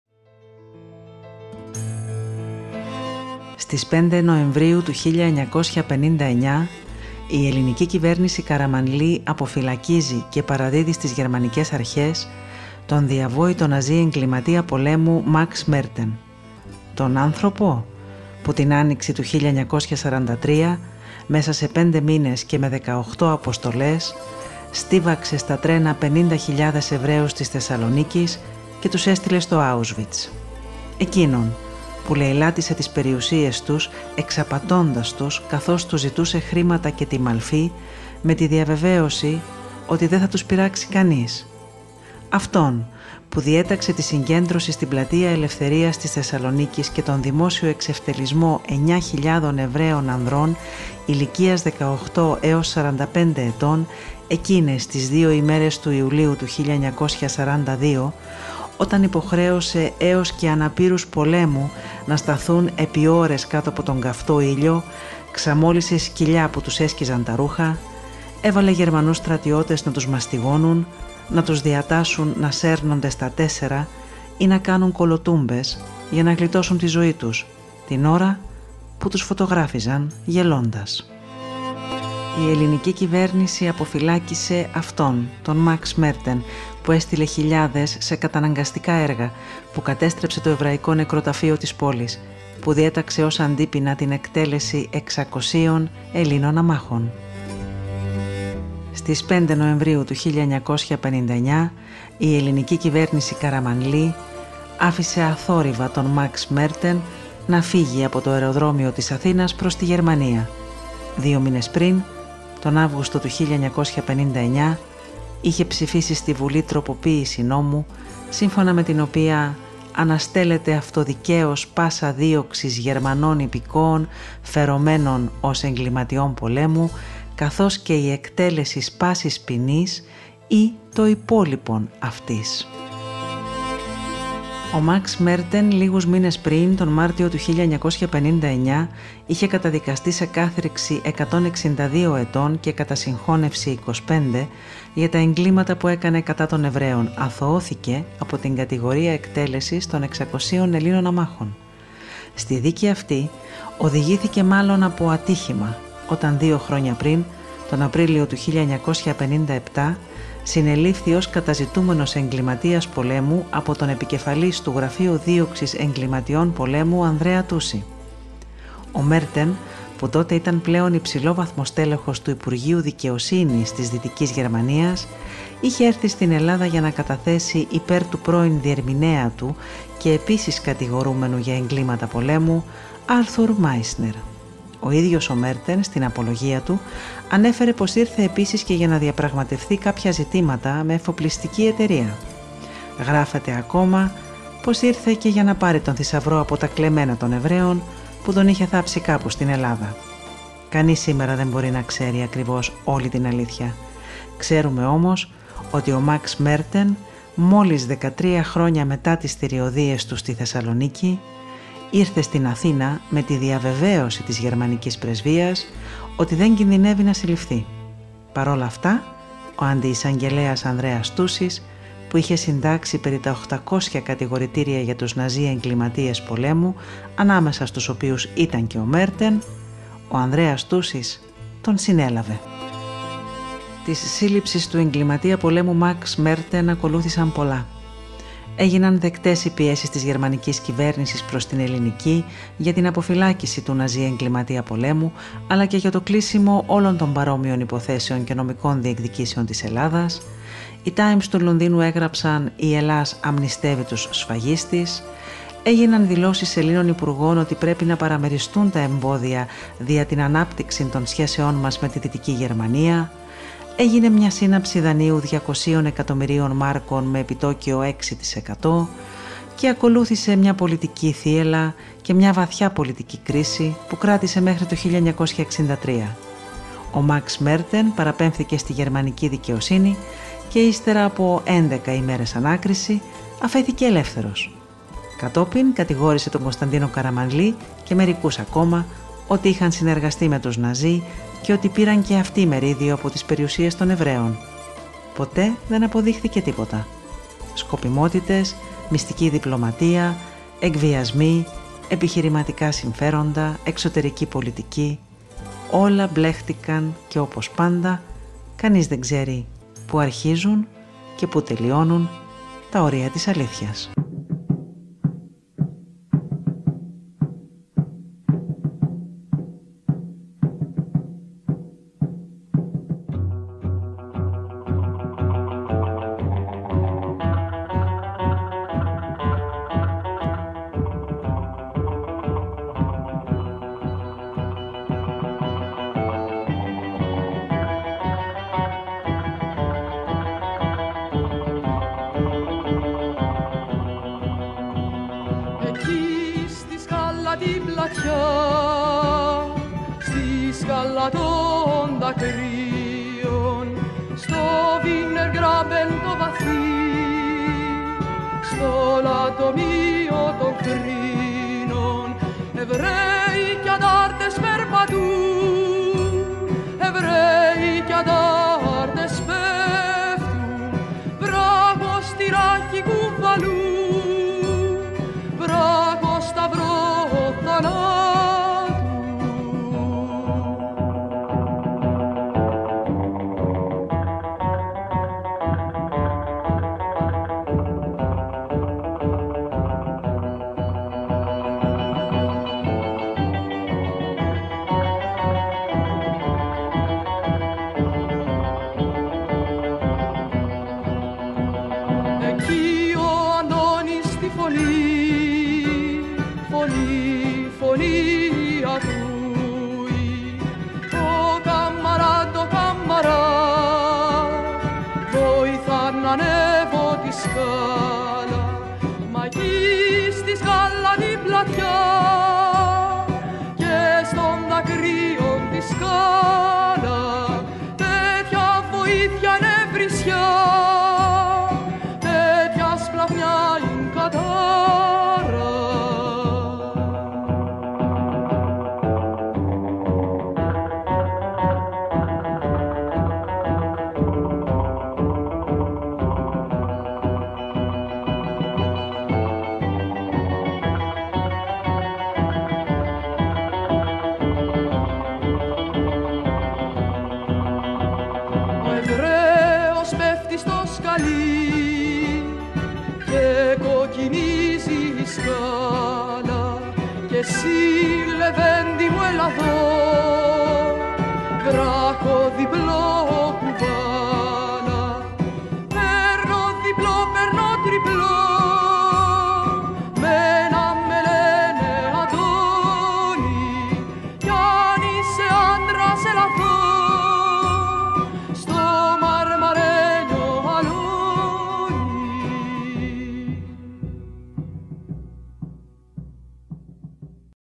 Εξ αφορμής: Υπόθεση Μέρτεν
Στις 5 Νοεμβρίου του 1959, η ελληνική κυβέρνηση Καραμανλή αποφυλακίζει και παραδίδει στις γερμανικές αρχές τον διαβόητο ναζί εγκληματία πολέμου Μαξ Μέρτεν. Τον άνθρωπο(;) που την άνοιξη του 1943 στοίβαξε στα τρένα 50.000 Εβραίους της Θεσσαλονίκης και τους έστειλε στο Άουσβιτς. Εκείνον που λεηλάτησε τις περιουσίες τους εξαπατώντας τους, που διέταξε τον δημόσιο εξευτελισμό 9.000 Εβραίων ανδρών ηλικίας 18-45 ετών στην πλατεία Ελευθερίας της Θεσσαλονίκης. Αυτόν, που έστειλε χιλιάδες σε καταναγκαστικά έργα, που κατέστρεψε το Εβραϊκό νεκροταφείο της πόλης, που διέταξε ως αντίποινα την εκτέλεση 600 Ελλήνων αμάχων.

Στις 5 Νοεμβρίου του 1959, η ελληνική κυβέρνηση Καραμανλή άφησε αθόρυβα τον Μαξ Μέρτεν να φύγει από το αεροδρόμιο της Αθήνας προς τη Γερμανία. Δύο μήνες πριν, τον Αύγουστο του 1959, ψηφίζει στη βουλή τροποποίηση νόμου σύμφωνα με την οποία «αναστέλλεται αυτοδικαίως πάσα δίωξις γερμανών υπηκόων φερομένων ως εγκληματιών πολέμου, καθώς και η εκτέλεσις πάσης ποινής ή το υπόλοιπον ταύτης».